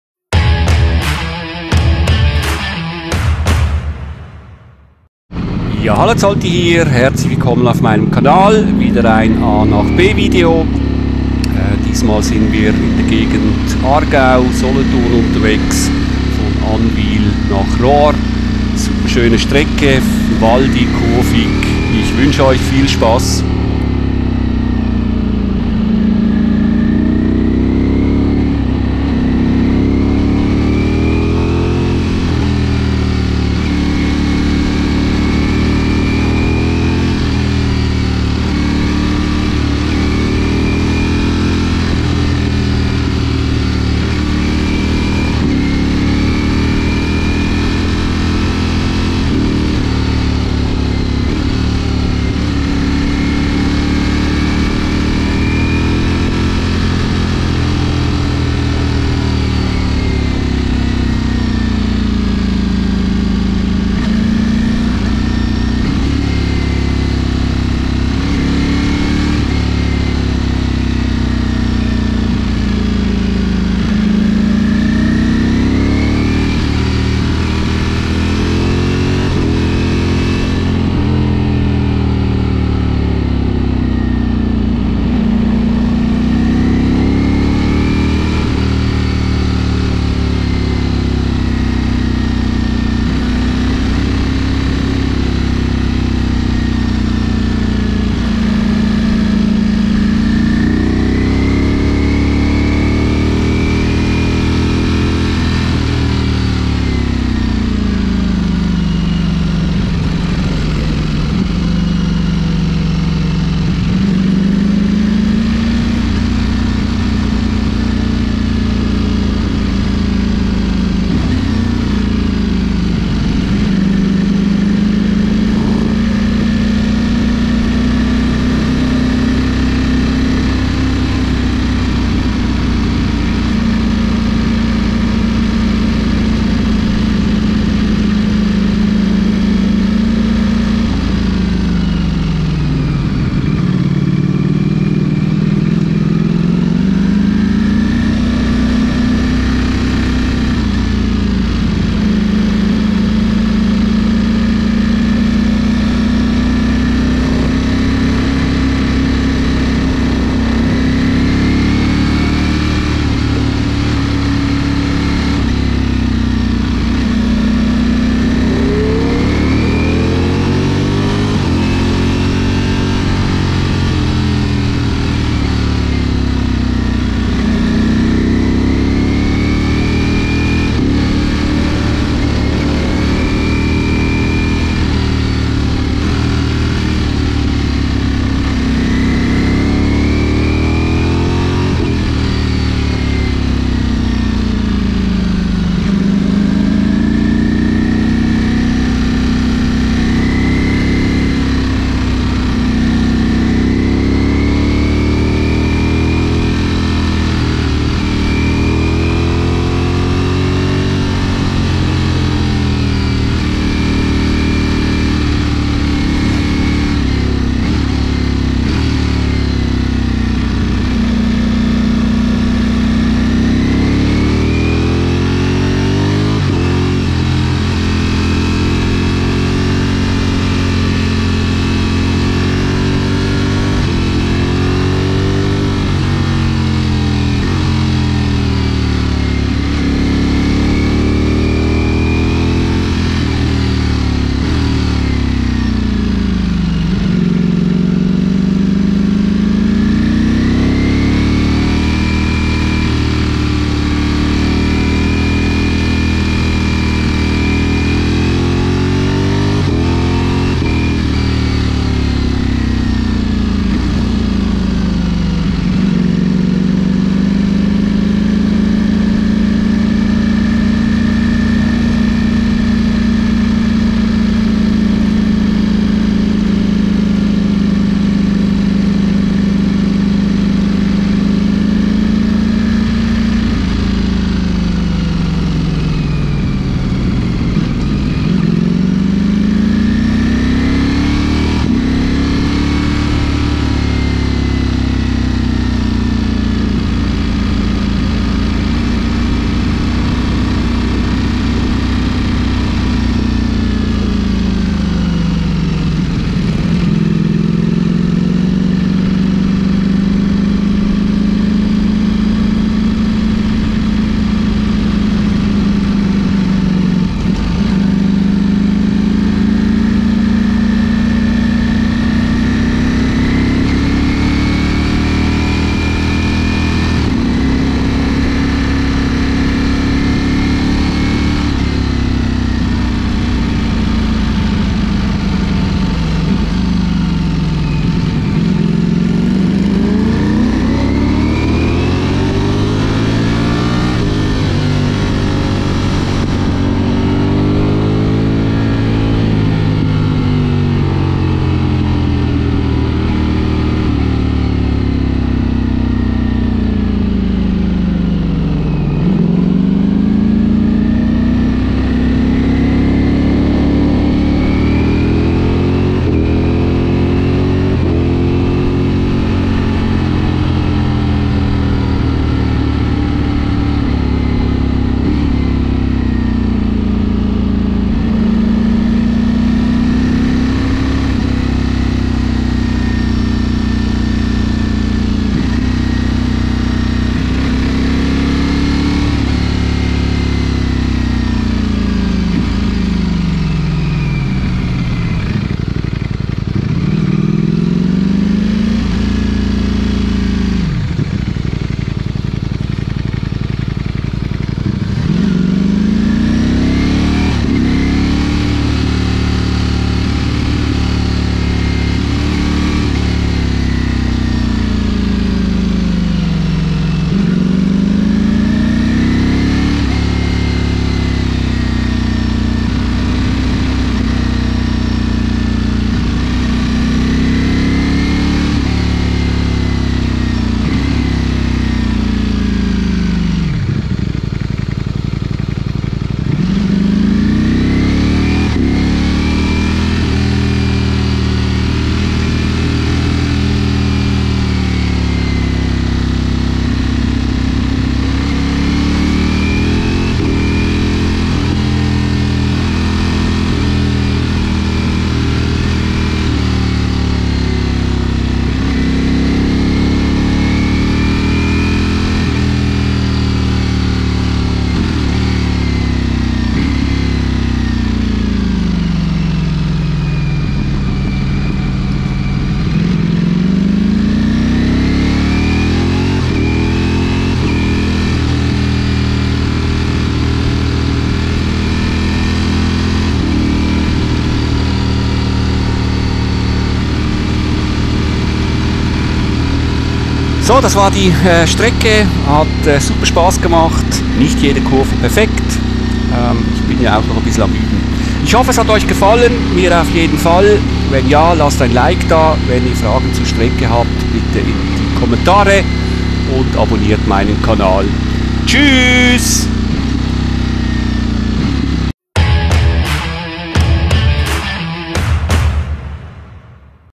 Ducati Streetfighter V4S 2021 / von A nach B Folge 3: Geissflueh; Anwil nach Rohr
Ich starte hier ein Reihe von A nach B Videos ohne gross gequatschte - darum halte ich jetzt meine Klappe. Ich hoffe ich kann Euch damit die Winterpause etwas versüssen :-) Lg Zolti

Region: Geissflueh (CH)
Strecke: 22,2 km; Anwil - Oltingen - Rohr